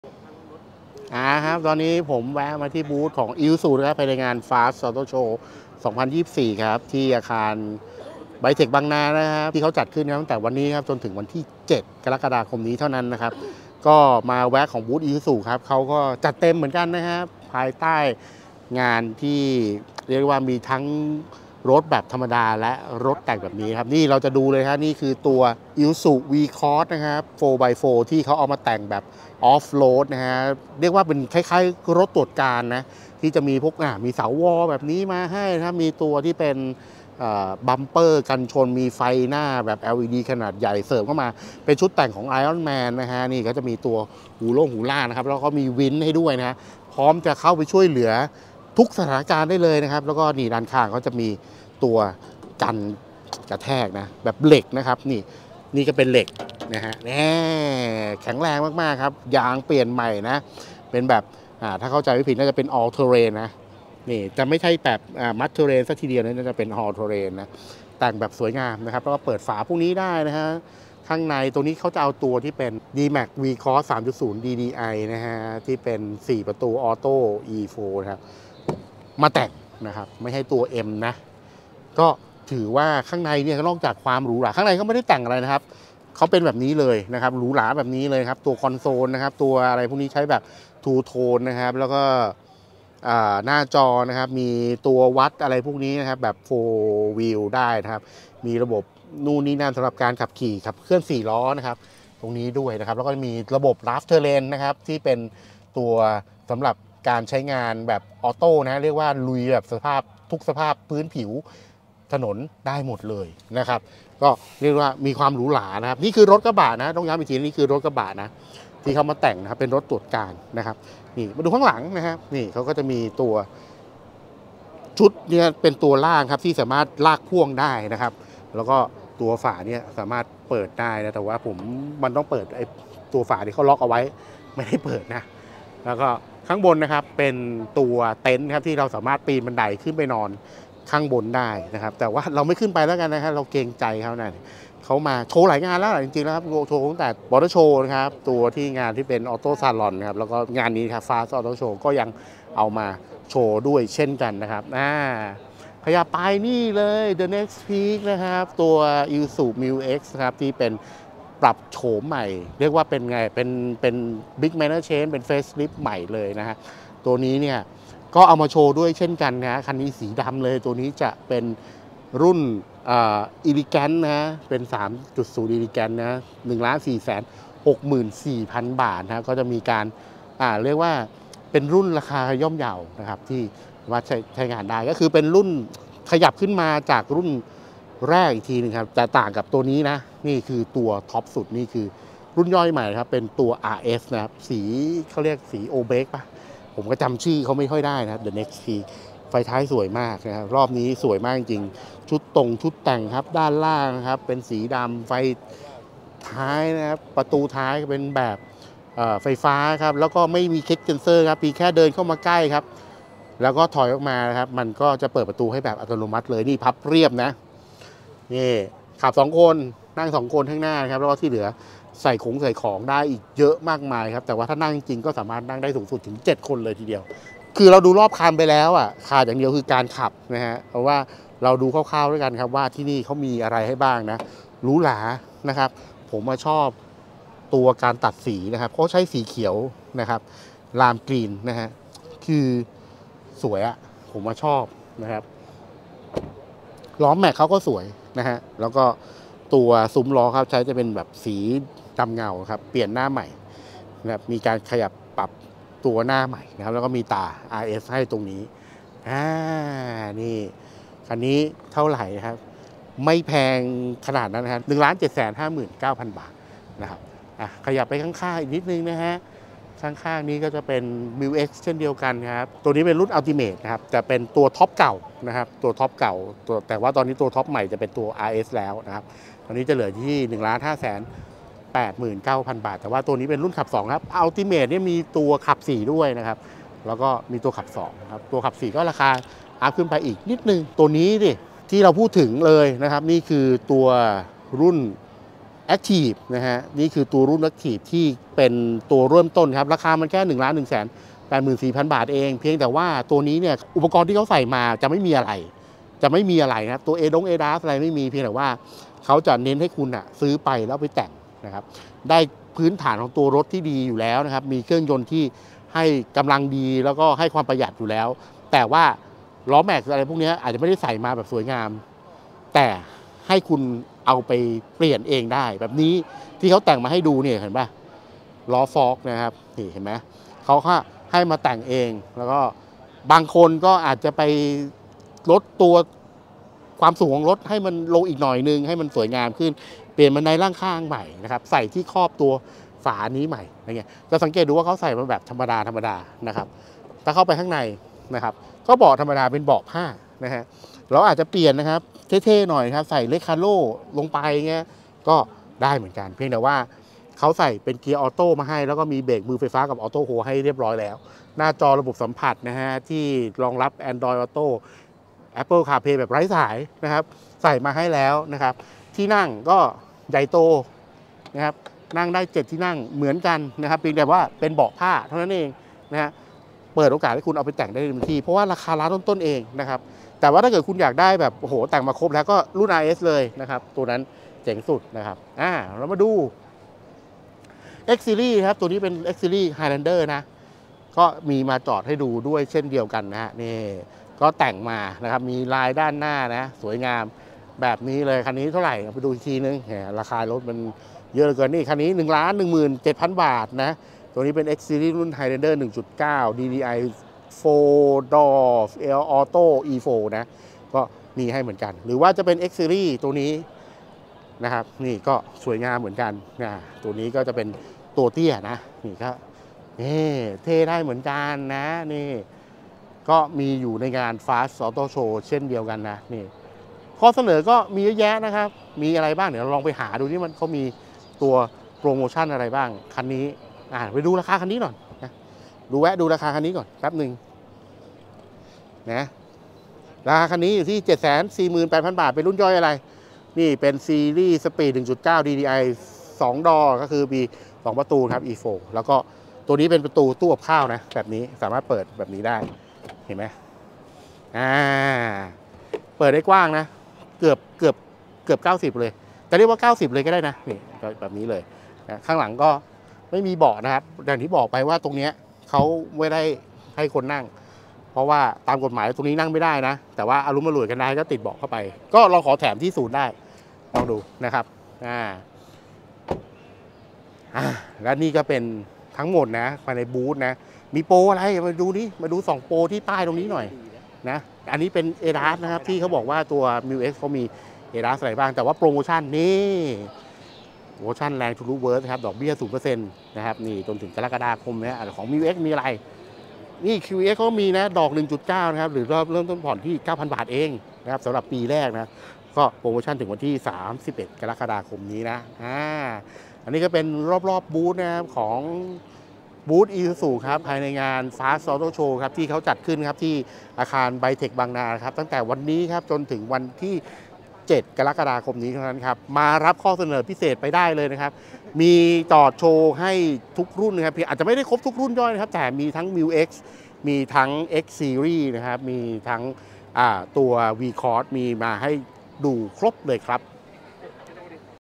เดินชมบูธ ISUZU ที่งาน FAST Auto Show Thailand 2024 นำหน้าด้วยรถใหม่ MU-X ครบทุกรุ่นย่อย
เริ่มแล้วกับงาน FAST Auto Show Thailand 2024 ที่จัดขึ้นระหว่างวันที่ 3-7 กรกฎาคมนี้ ที่อาคารไบเทค บางนา โดยคลิปนี้จะพาไปเดินชมรอบบูธ ISUZU ที่นำทัพหน้ามาด้วยรถใหม่ 2024 ที่เพิ่งเปิดตัวอย่าง MU-X ที่มาครบทุกรุ่นย่อย ไม่ว่าจะเป็น RS, Ultimate, Elegant และ Active รายละเอียดเป็นอย่างไรบ้าง ติดตามชมกันได้เลย